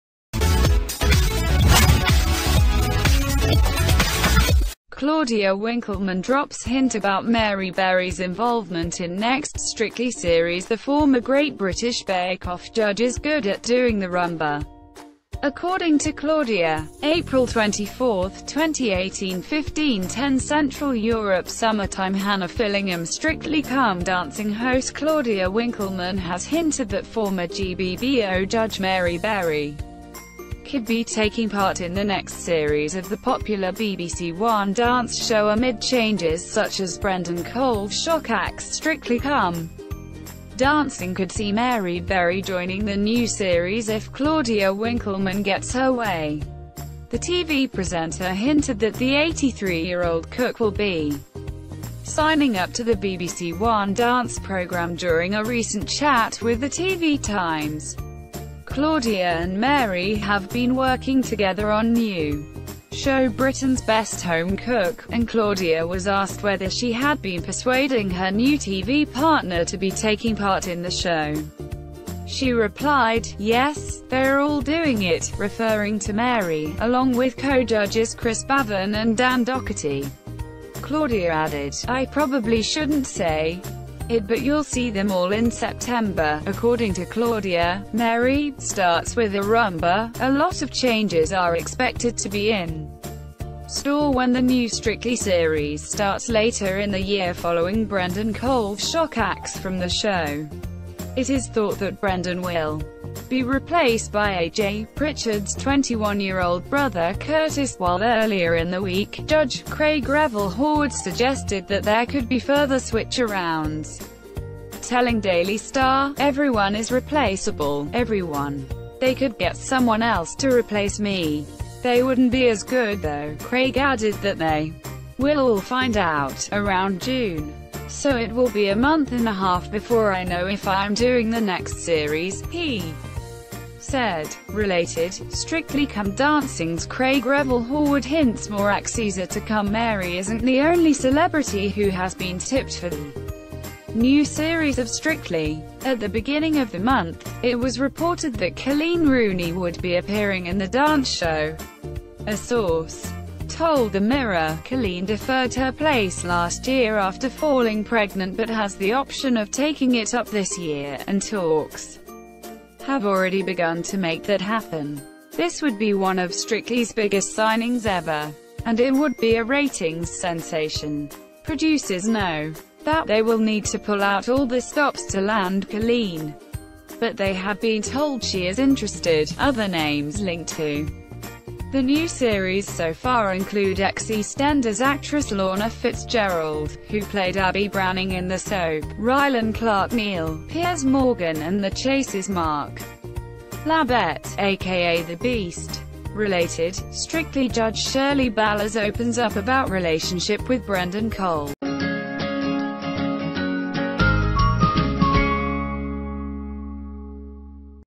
Claudia Winkleman drops hint about Mary Berry's involvement in next Strictly series
► Claudia Winkleman drops hint about Mary Berry's involvement in next Strictly series The former Great...
►  Show Tv UK Channel dedicated to sharing the latest news around the world.